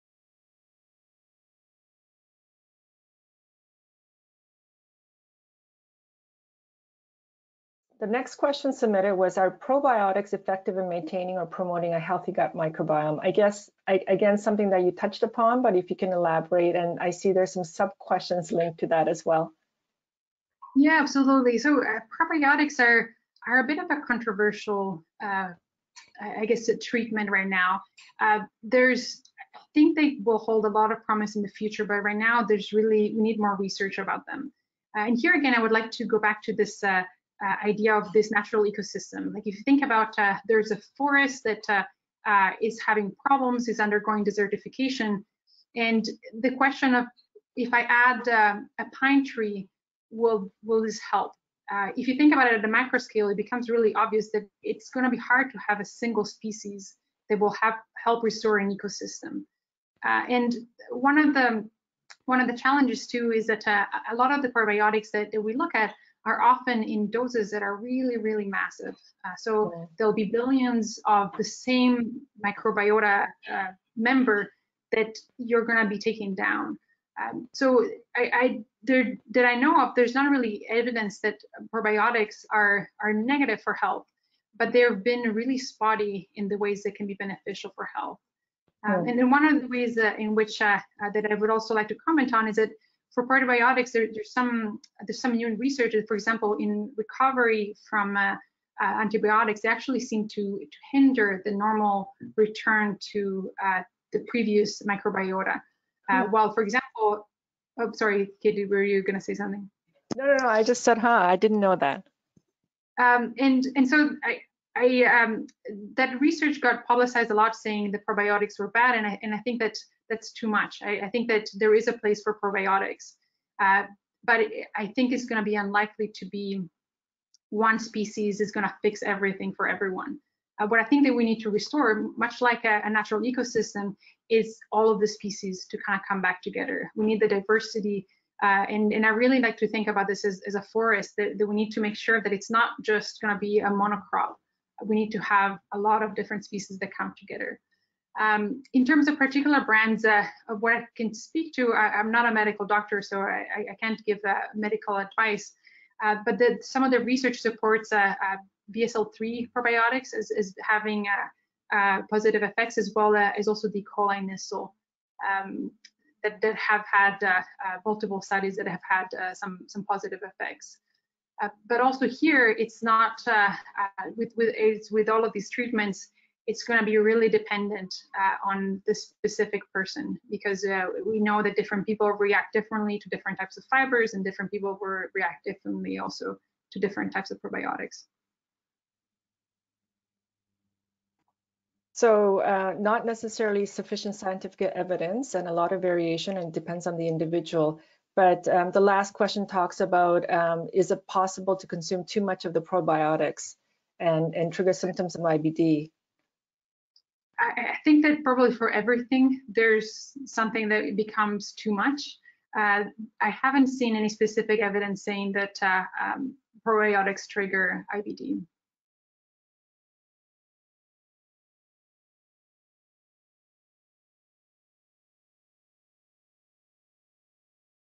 Can Probiotics Promote A Healthy Gut Microbiome?
Watch this video to learn if probiotics are effective at maintaining and promoting a healthy gut microbiome in IBD.

Speaker: Dr. Carolina Tropini is an Assistant Professor at The University of British Columbia in the School of Biomedical Engineering and Department of Microbiology and Immunology.